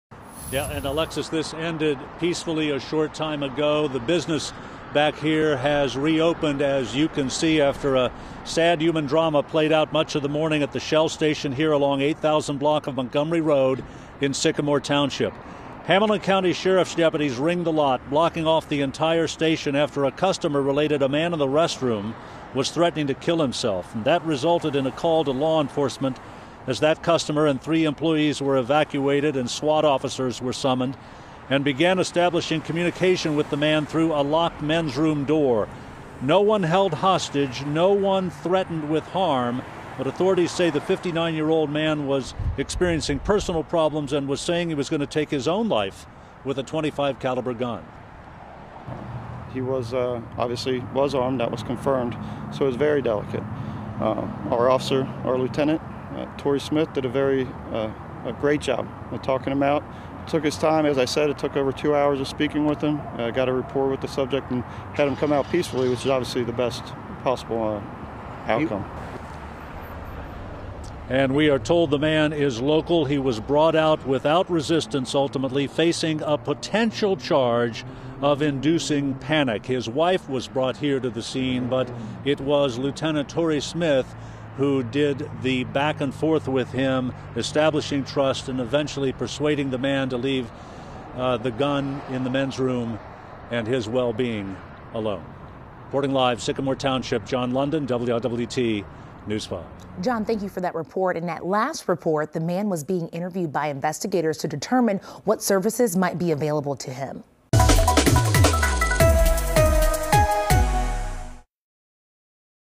Gas station standoff ends peacefully in Sycamore Township
Armed man barricades self in bathroom